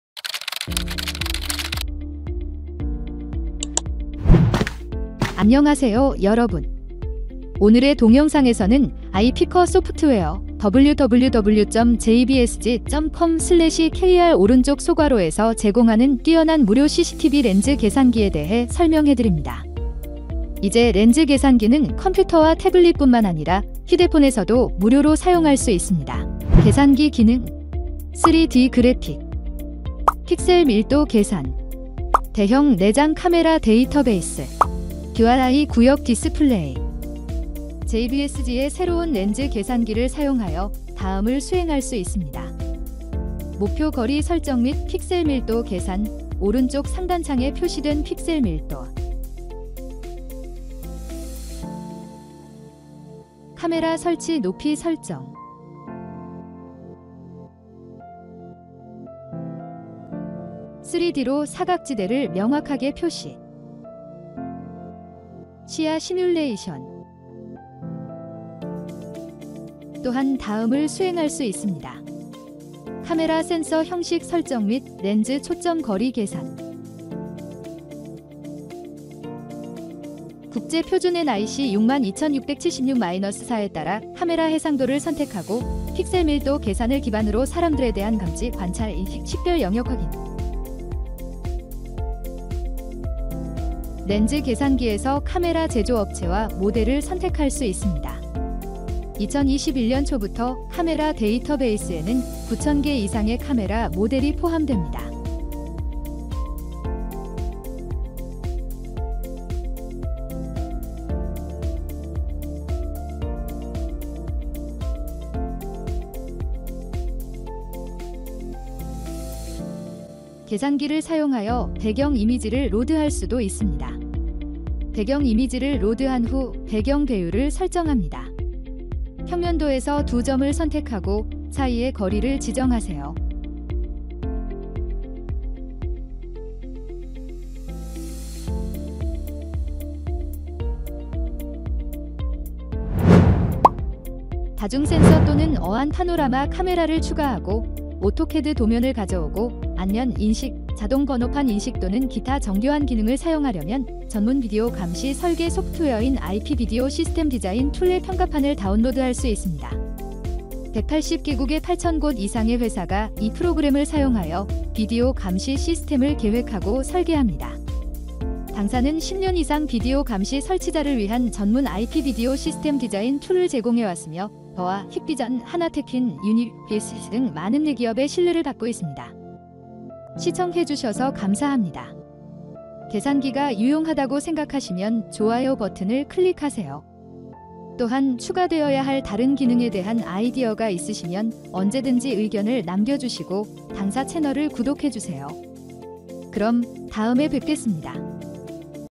CCTV 렌즈 계싼기
안녕하세요, 여러분! 오늘의 동영상에서는 Ipica Software 제공하는 뛰어난 무료 CCTV 렌즈 계산기에 대해 설명해드립니다.
계산기 기능:
• 3D 그래픽
• 픽셀 밀도 계산
• 대형 내장 카메라 데이터베이스
• DORI 구역 디스플레이